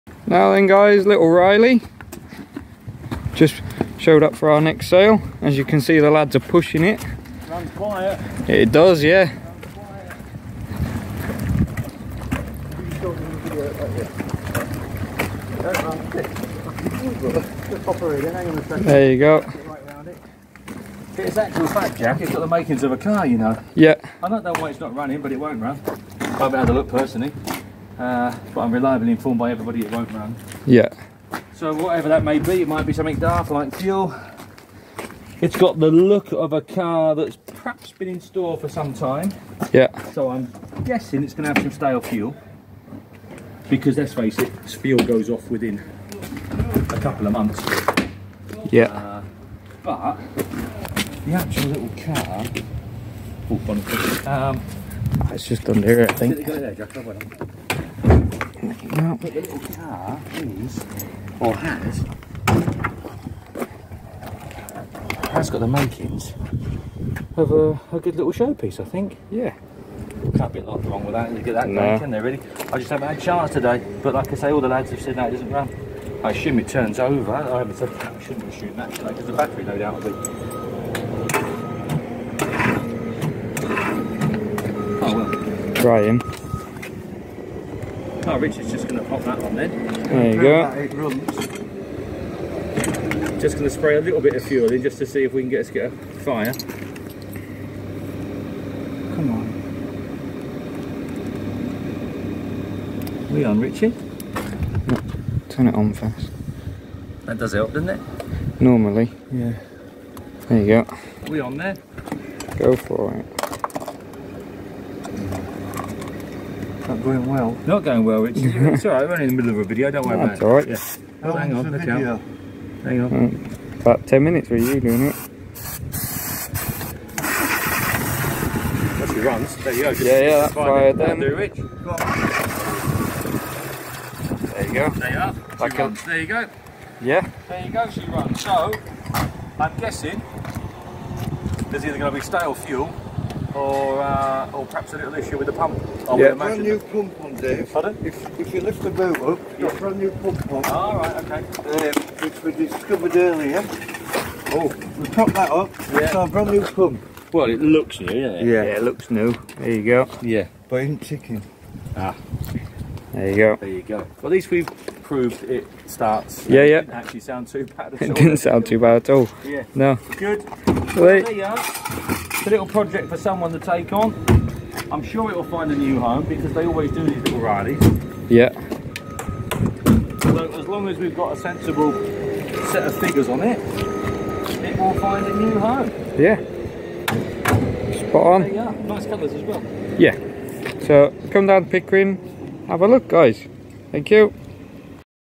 1967 RILEY KESTREL 1100 | MATHEWSONS CLASSIC CARS | AUCTION: 4, 5 & 6 SEPTEMBER 2024
1967 RILEY KESTREL 1100 | MATHEWSONS CLASSIC CARS | AUCTION: 4, 5 & 6 SEPTEMBER 2024

Join Derek, Dave, Paul and everyone at Mathewsons Classic Cars for our live online classic car, motorcycle and memorabilia auction on 4, 5 & 6 September 2024.